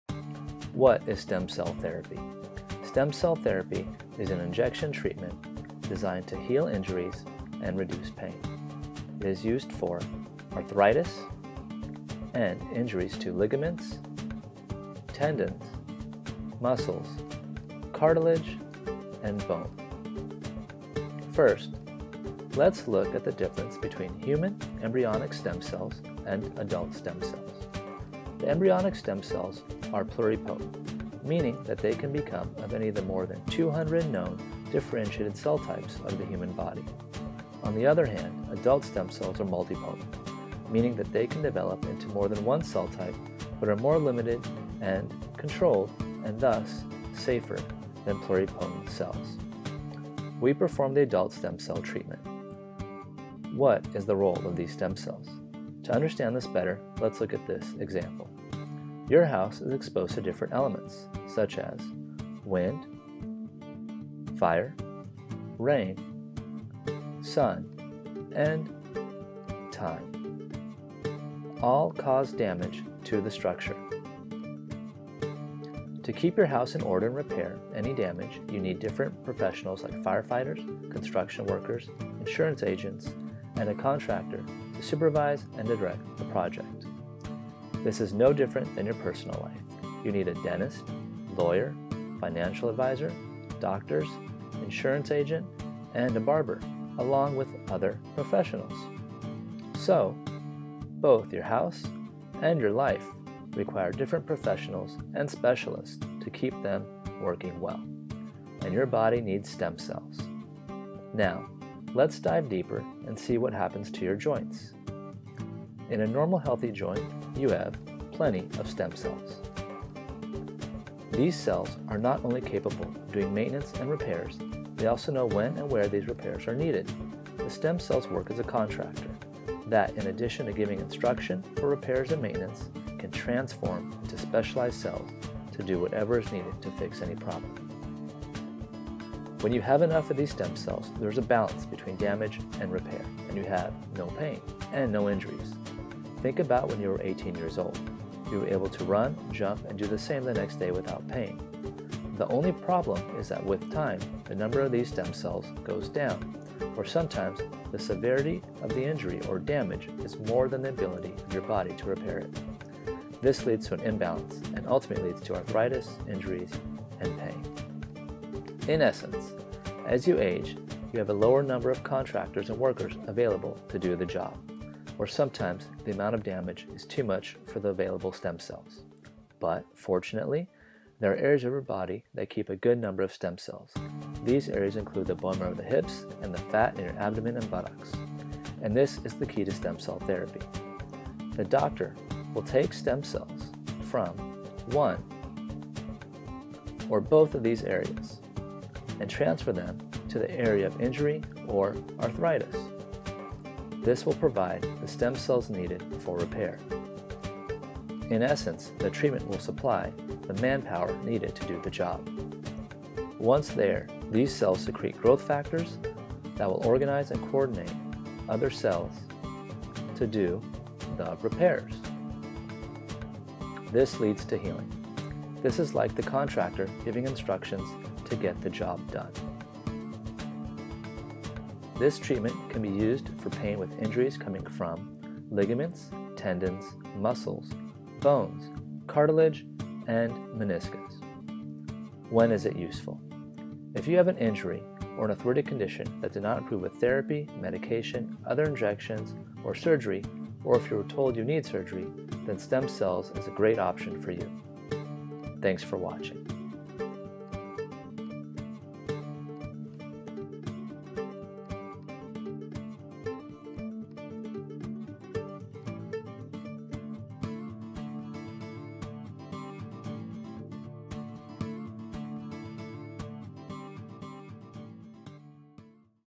What is stem cell therapy (Orthobiologics)
Dr. Barreto works at Regenomics in Orlando, Florida. Orthobiologics are used to treat musculoskeletal injuries and pain.
This animated video explains in an easy way why we need stem cells and how it helps you heal from injuries. Thanks to Dr. Bobby Nourani for narrating this video.